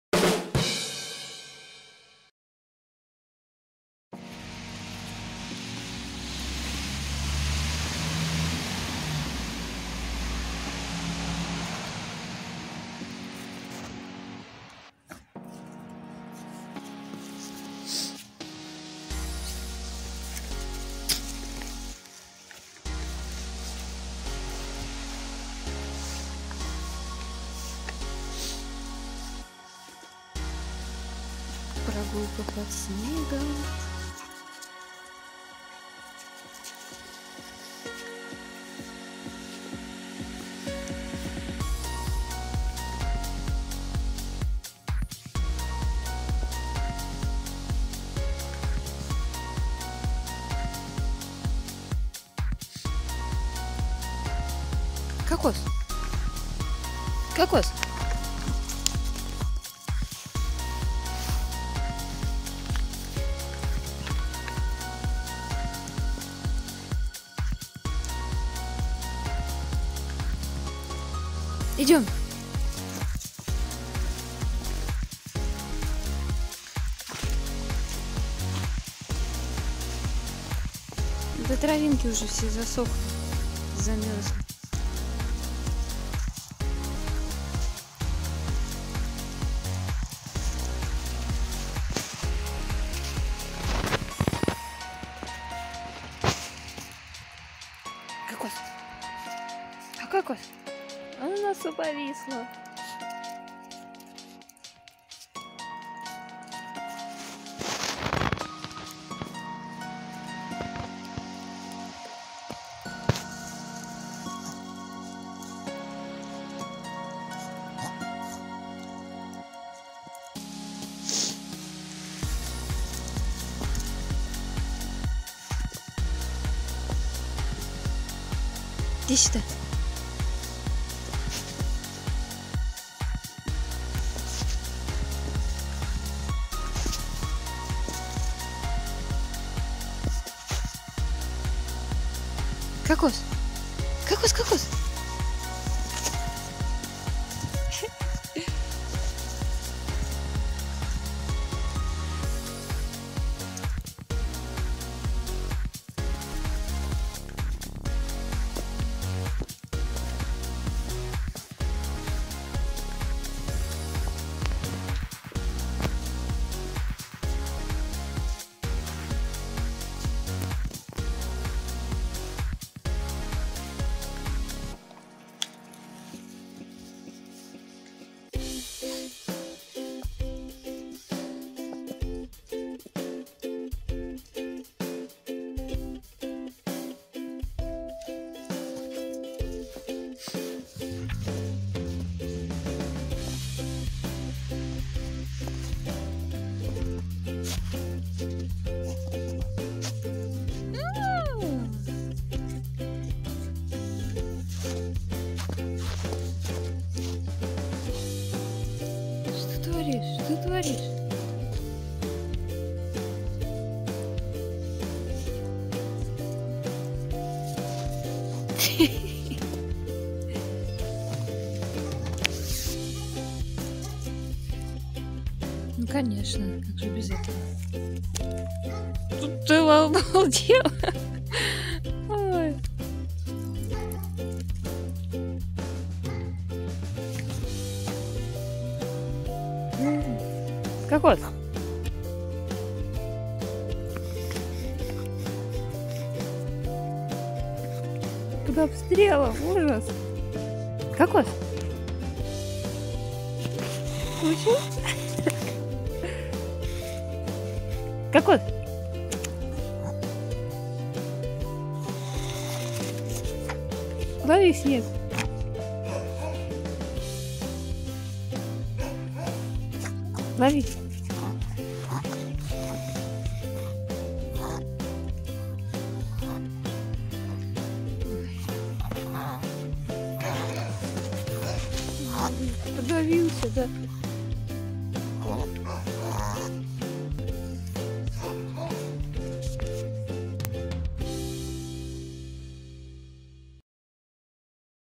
БЕЗЗАБОТНЫЙ ЗИМНИЙ ДЕНЬ / A CAREFREE WINTER DAY
Кокос гуляет и играет.

Coconut walks and plays.